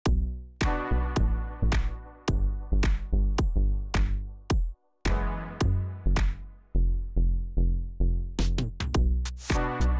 Eden Karzev, Başakşehir'de
Başakşehir Eden Karzev transferini resmen açıkladı.